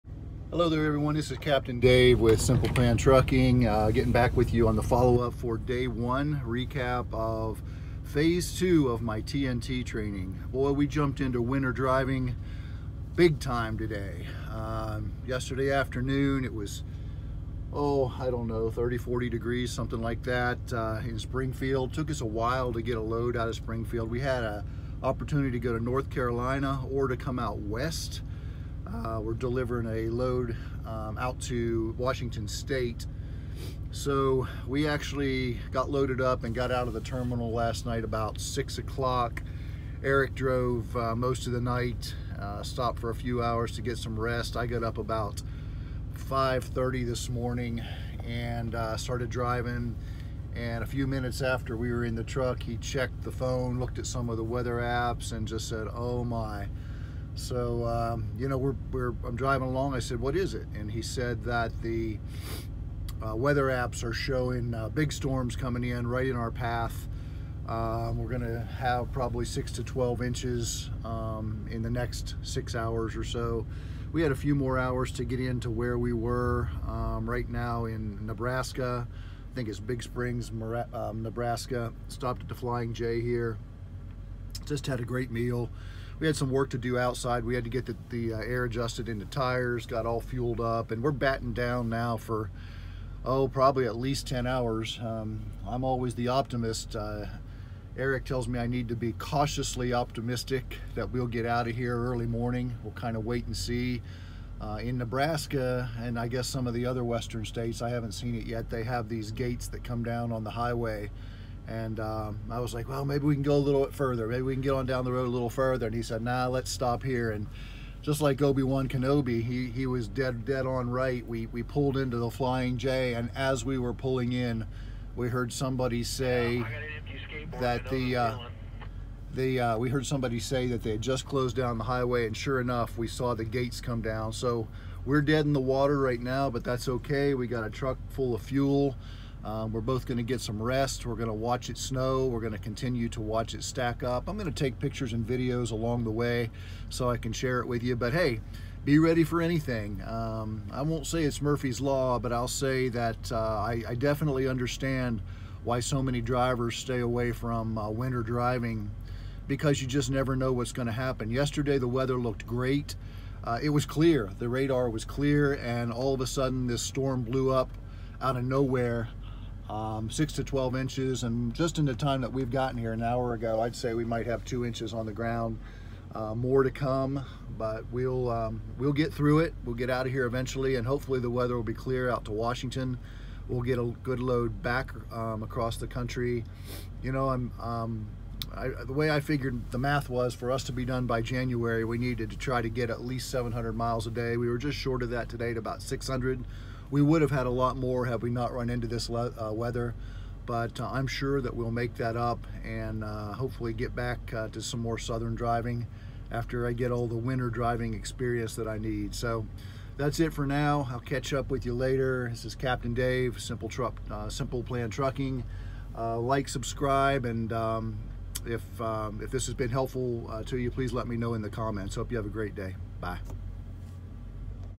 Winter Has Come!
Shut down on I-80 with big winter storm approaching.